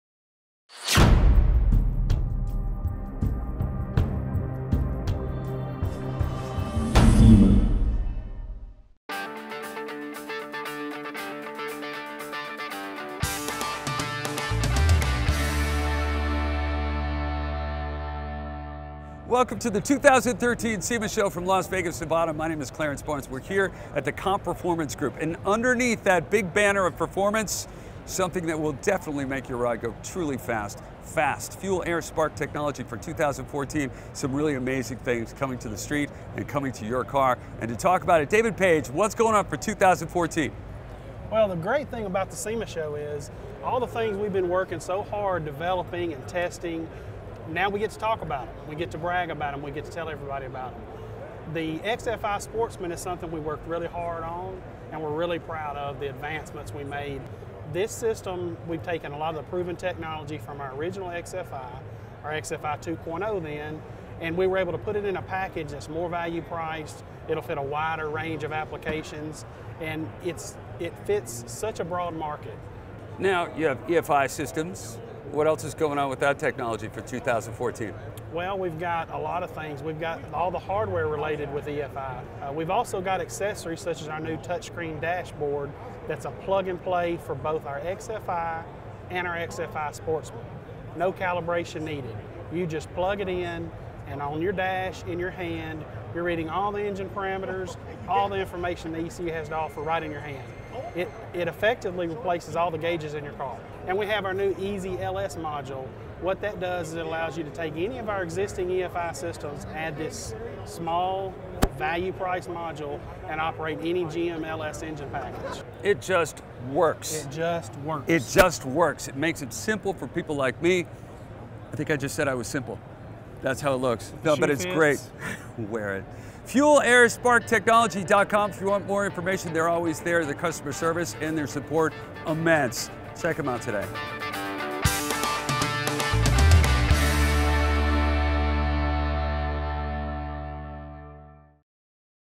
FAST 2013 SEMA Show New Products
Clarence Barnes visits with David Page of FAST (Fuel Air Spark Technology) at the 2013 SEMA Show in Las Vegas to discuss the latest in EFI technoogy for racers & performance enthusiasts.